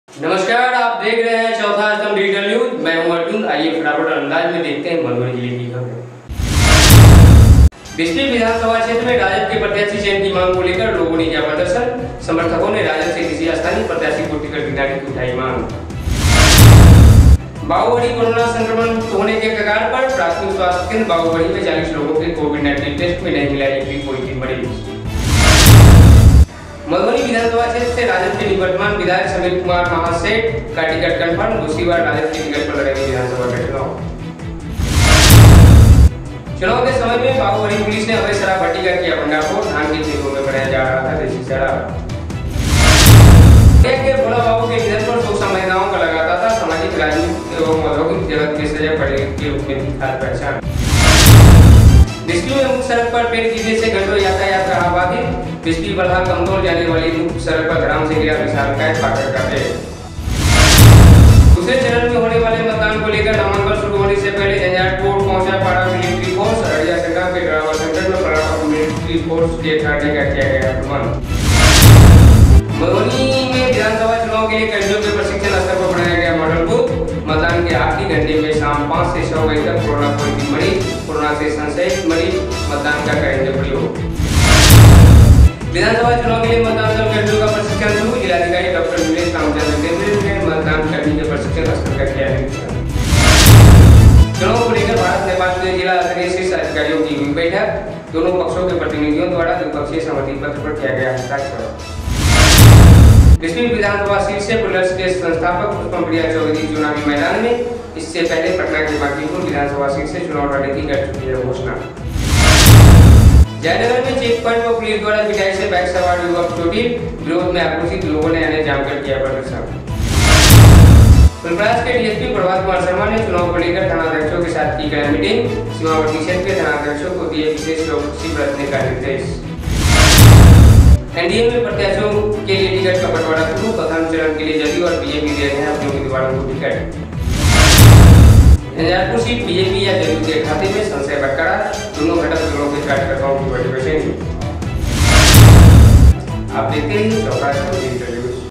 6 अक्टूबर 2020 की मधुबनी जिले की खबरें।
fatafata news, # madhubani